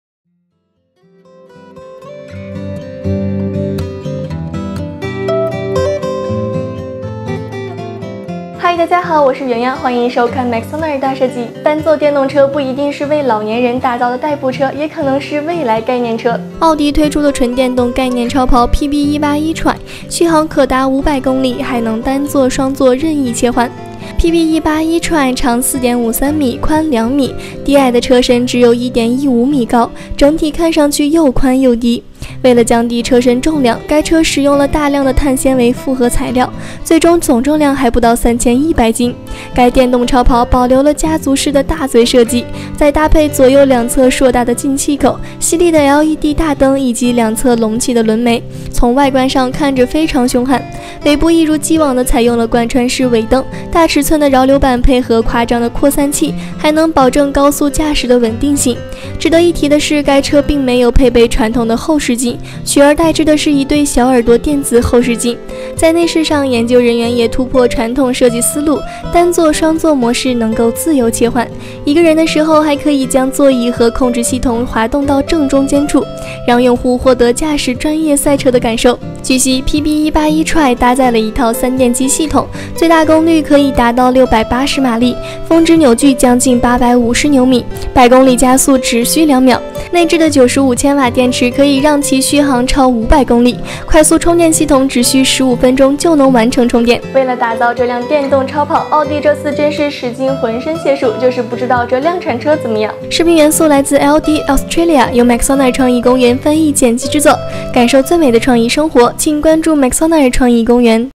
最凶悍的奥迪电动超跑，单双座任意切换，百公里2秒，续航500公里
单座电动车不一定是为老年人打造的代步车，也可能是未来概念车。奥迪推出的纯电动概念超跑PB 18 e-tron，续航可达500公里，还能单座双座任意切换